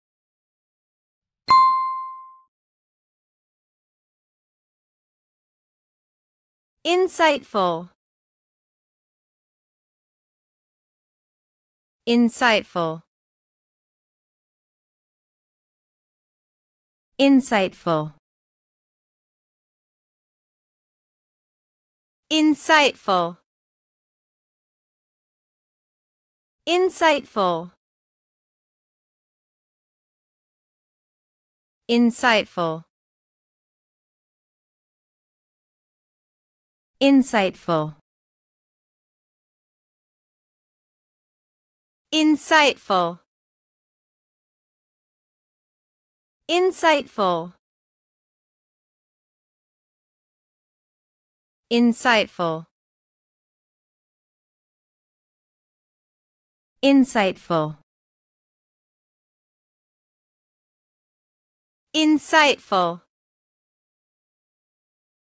How to pronounce Insightful in English correctly
This video shows you how to say (Insightful). Listen and learn the English pronunciation of Insightful.

𝗗𝗲𝗳𝗶𝗻𝗶𝘁𝗶𝗼𝗻:
Insightful: Has a deep understanding